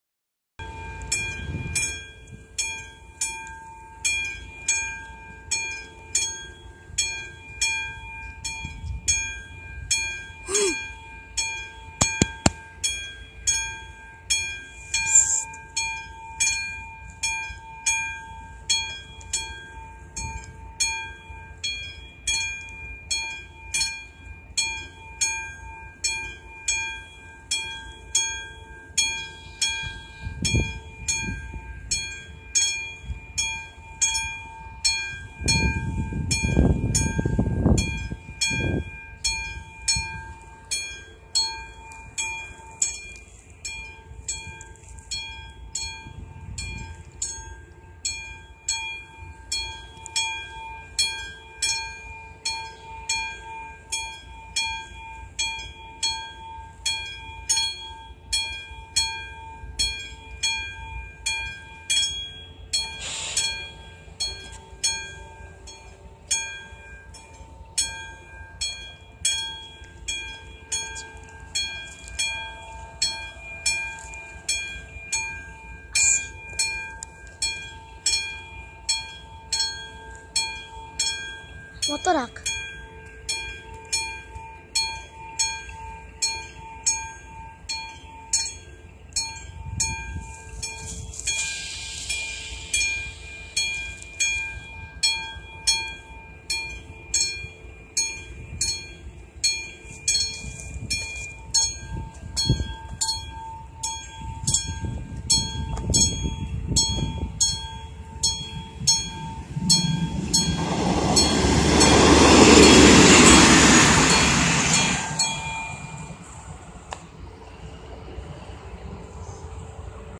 Železniční přejezd Trmice 1
železniční přejezd se nachází mezi stanicemi Trmice - Koštov